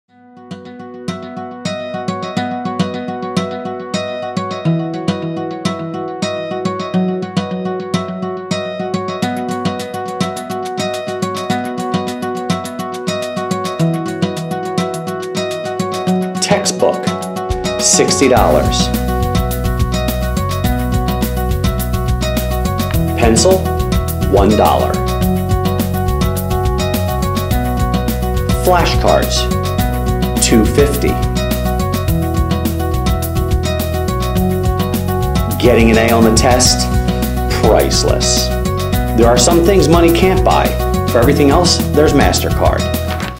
priceless bella tierno
This video is about advertisement. For me the best thing we did was come up with the objects we had to use. If we had to do this again we would do a different theme because this one is kind of common. But it was fun to do.